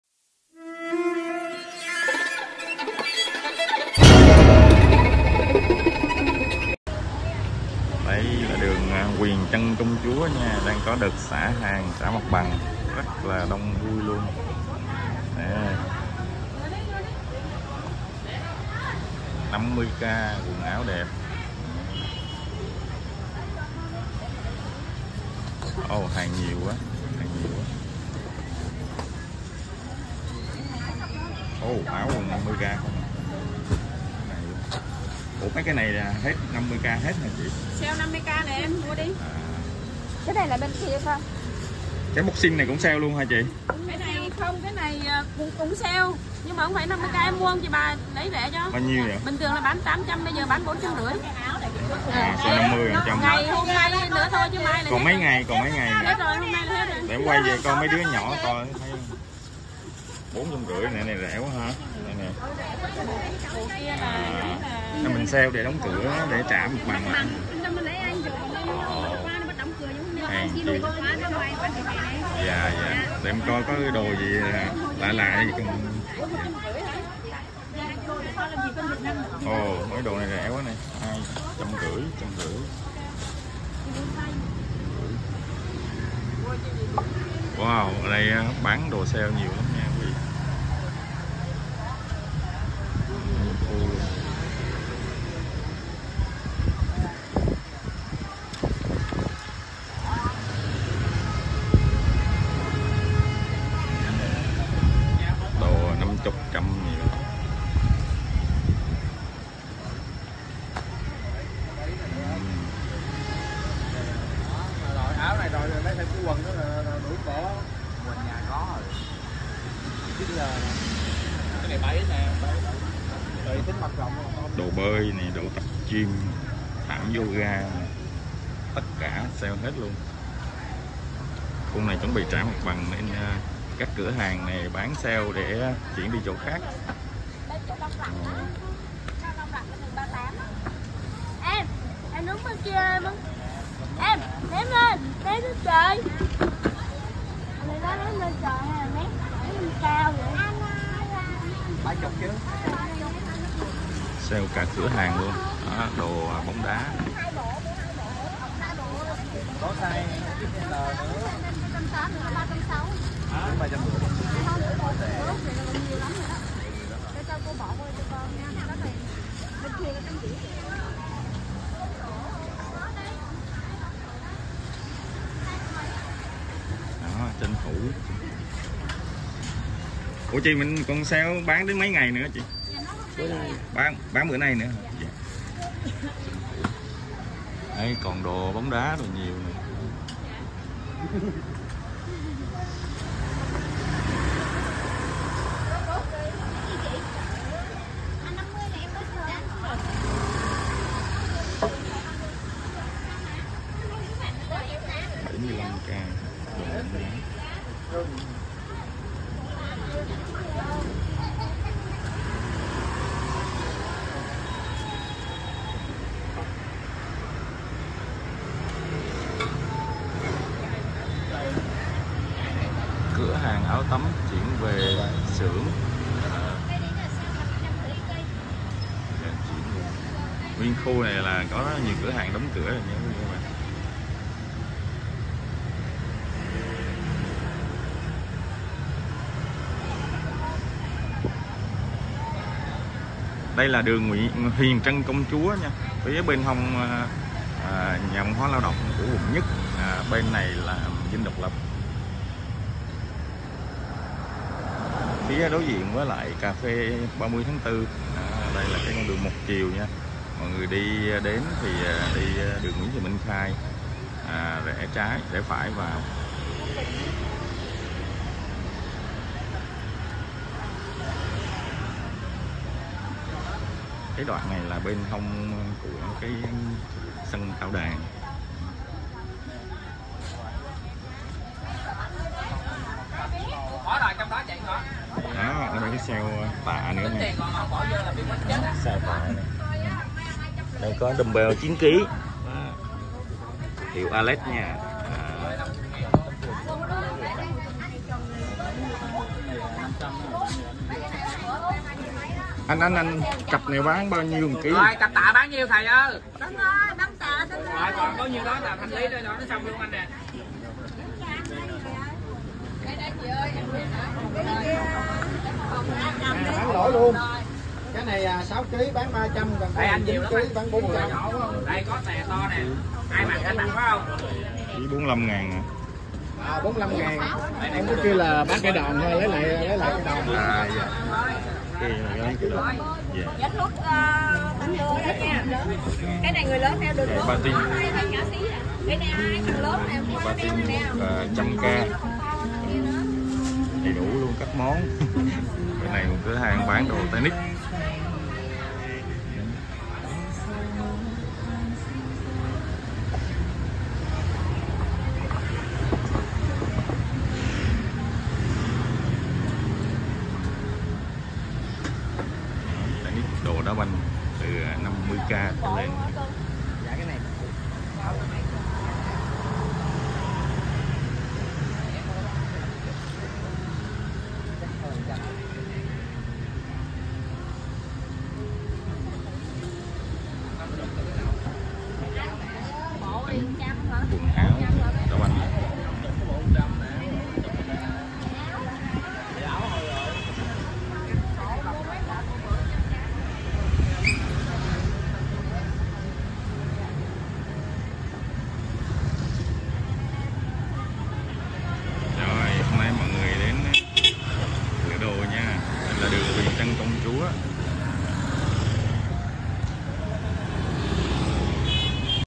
Xả hàng mạnh, dẹp khu chợ đồ thể thao Huyền Trân Công Chúa Q1 gần chợ Bến Thành
NGÀY CUỐI | Xả hàng mạnh, dẹp khu chợ đồ thể thao Huyền Trân Công Chúa Q1 gần chợ Bến Thành.